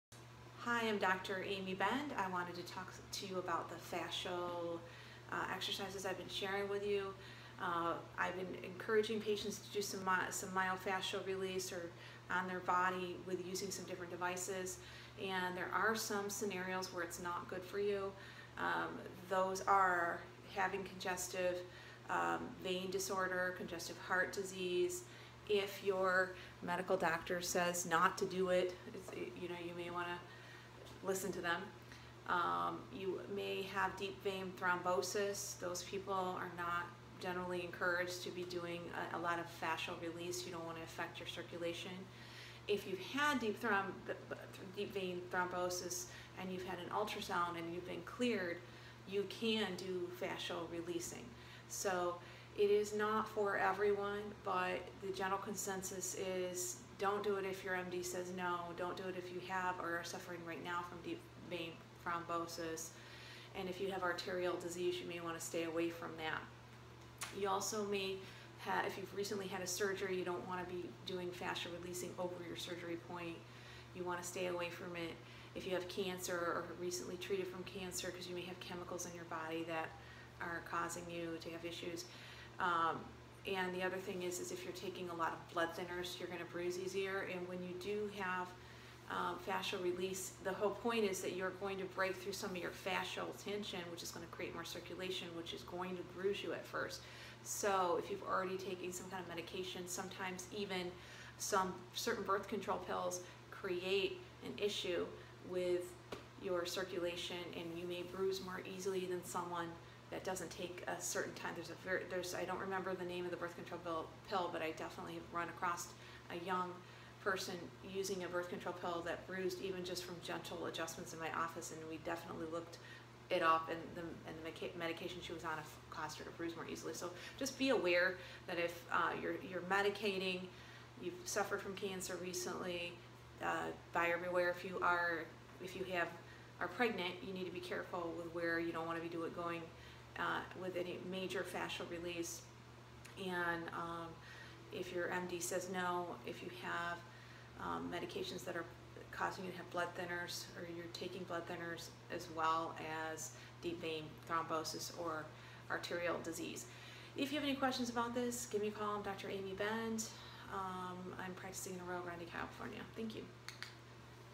When not to do fascia release?| Fascia Release risk| when Shouldnt I do Fascia Release?
Dr. Amie Bend DC discusses when its not appropriate for you to do Fasica release with a device.   Dr. Amie Bend has been in practice since 1998.